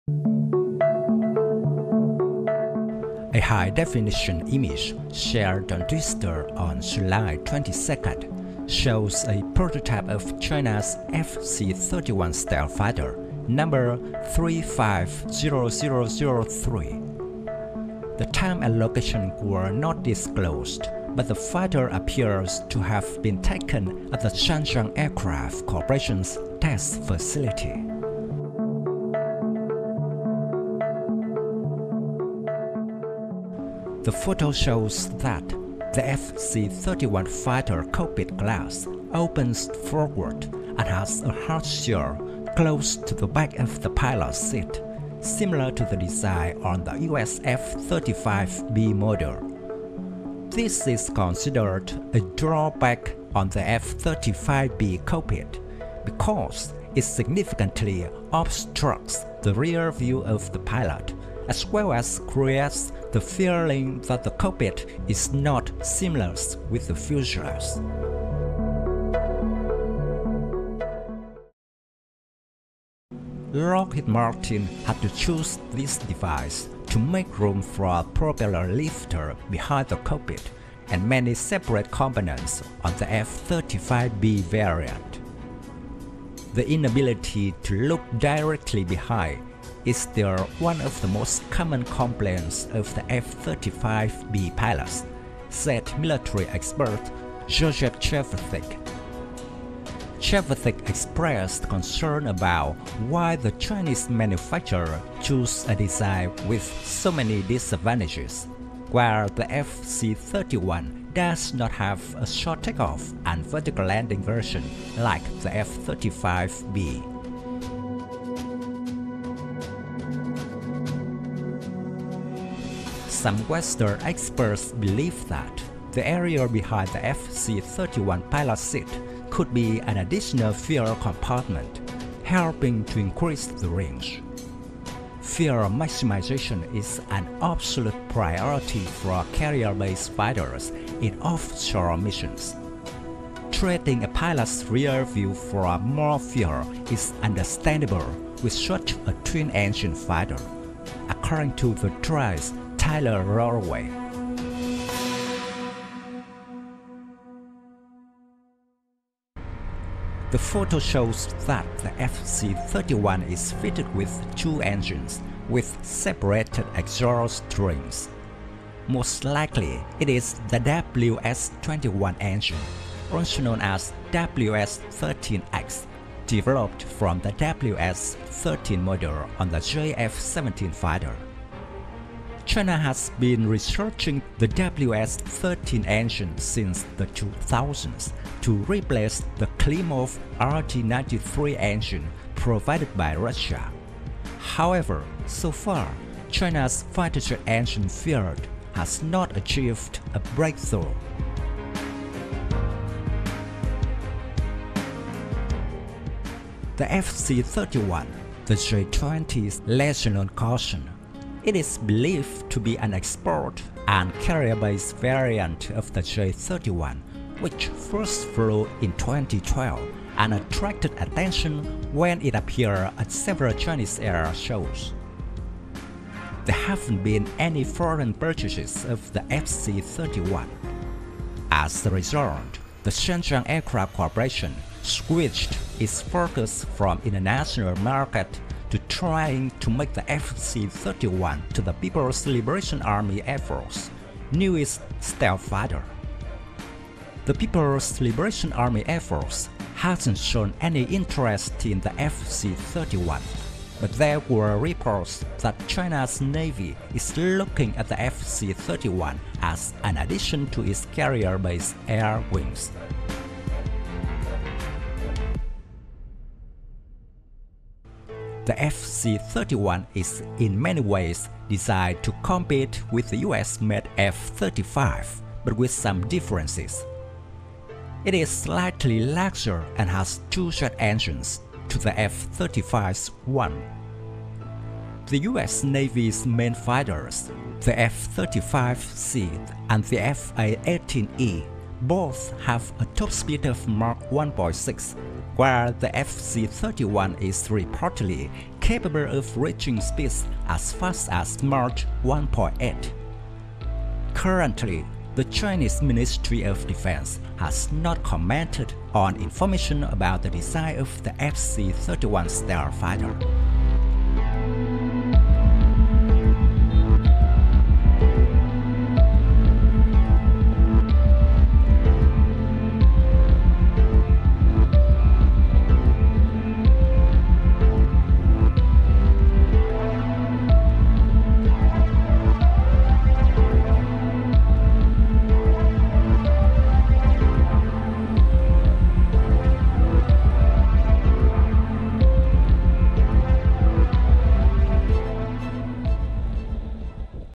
China's FC-31 Carrier-Based Fighter Exposes The Same Disadvantage As The US F-35B?
A high-definition image shared on Twitter on July 22 shows a prototype of China's FC-31 stealth fighter, number 350003. The time and location were not disclosed, but the fighter appears to have been taken at the Shenyang Aircraft Corporation's test facility.

Thank you very much.
Best regards
Dung Tran.